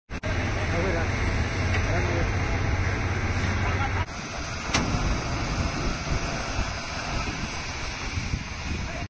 Small role, huge impact.
Small role, huge impact.                                   4 loco hauled coaches export to Africa from Raebareli, UP.